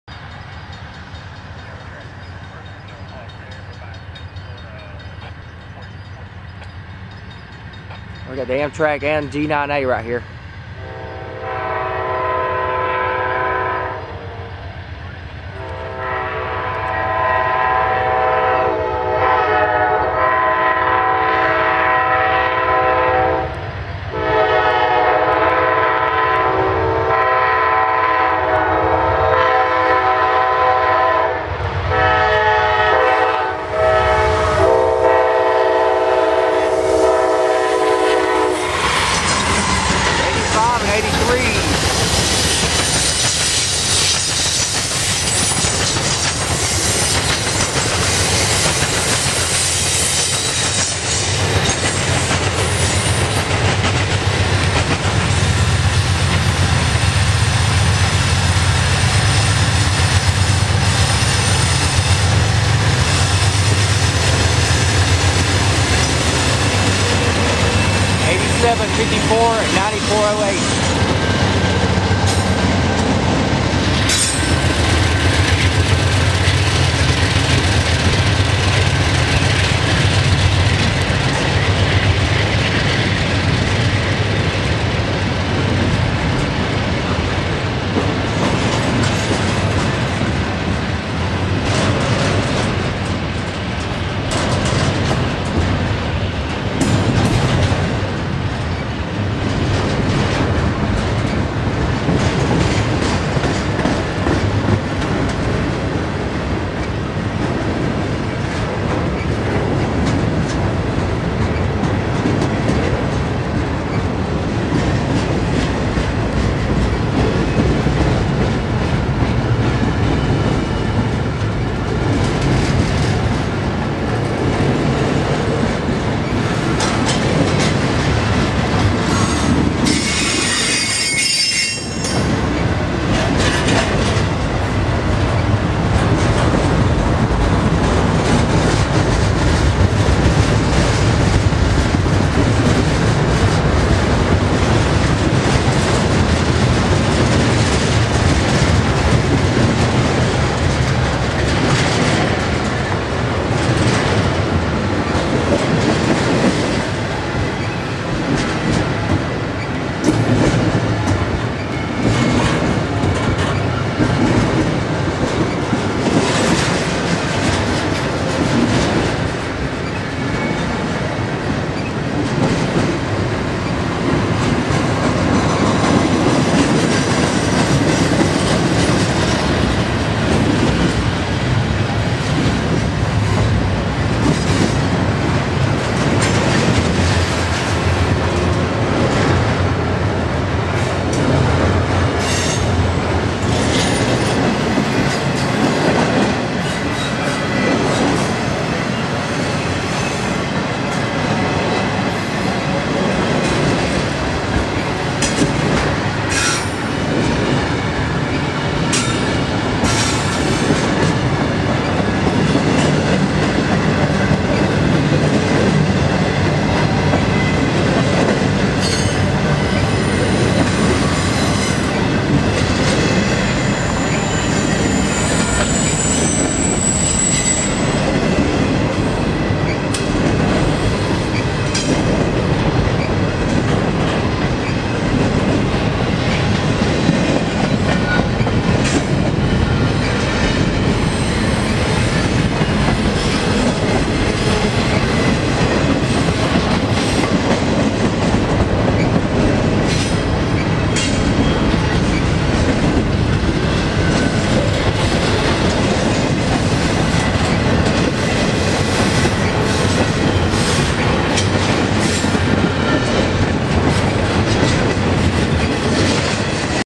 The Amtrak Crescent #20 w/ Awesome Crew & NS G9A SB in Austell,Ga 09-23-2012© (16x9)
7:58PM in Austell,Ga is the Amtrak Crescent #20 with awesome crew and NS G9A Southbound local intermodal...

Recorded by Gene and Tommy Hale on September 23, 2012